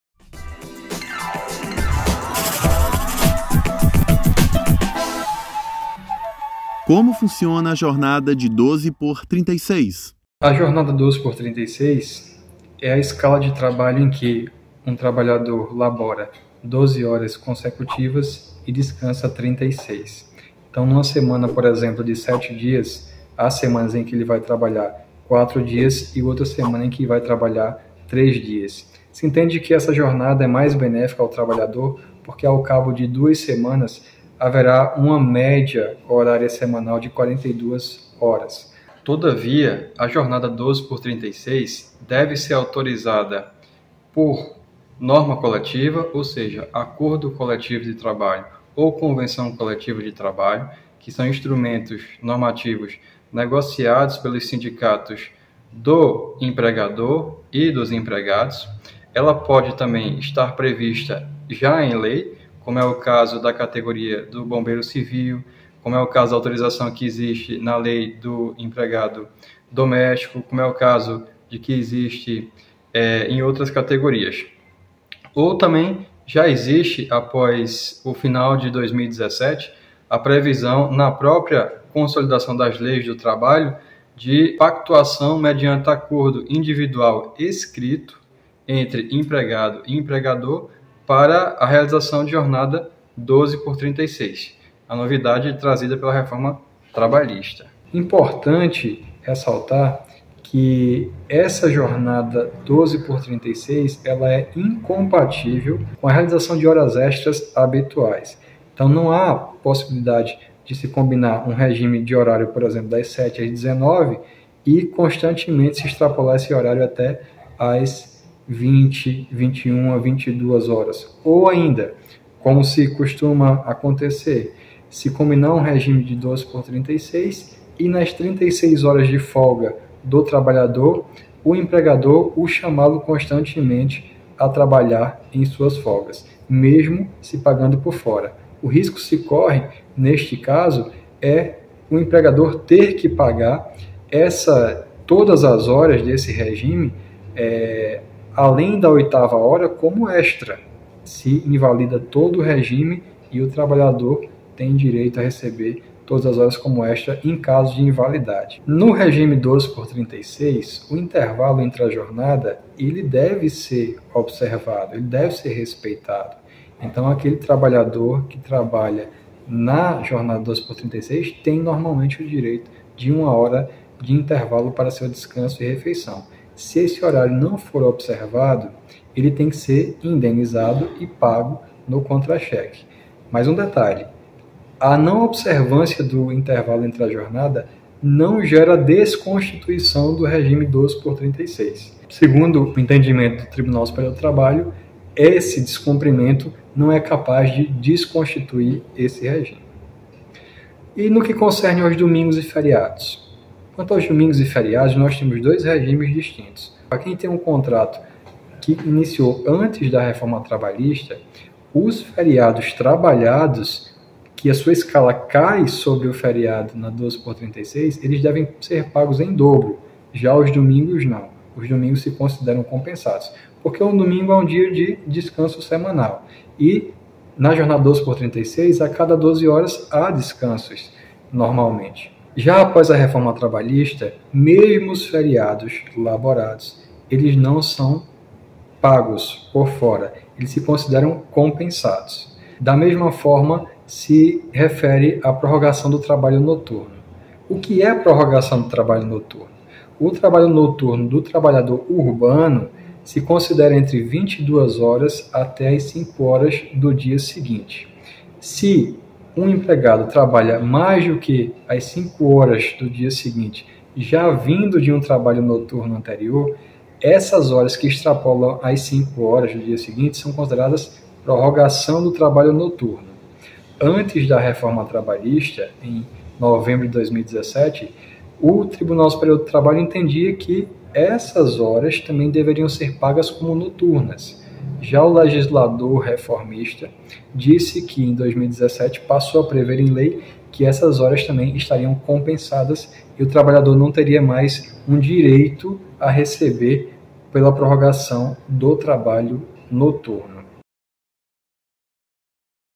Como funciona a jornada de 12x36? | Explicando Direito
A jornada de 12x36 é uma realidade para muitas categorias profissionais. E neste episódio do Explicando Direito o juiz do trabalho Hantony Cássio Ferreira da Costa vai dizer o que é essa jornada, quais as regras e como fica o pagamento por dias trabalhados em feridos, domingos e no horário noturno.

*O vídeo reflete o entendimento do magistrado.

Índice do vídeo:
00:00 Introdução
00:09 Jornada de 12x36
01:39 Horas extras na jornada de 12x36
02:38 Intervalos na jornada de 12x36
03:20 Domingos e feriados na jornada de 12x36
04:02 Horário noturno na jornada de 12x36

Ficha técnica:
Produção: José Anchieta
Edição: José Anchieta e Luciano Meletti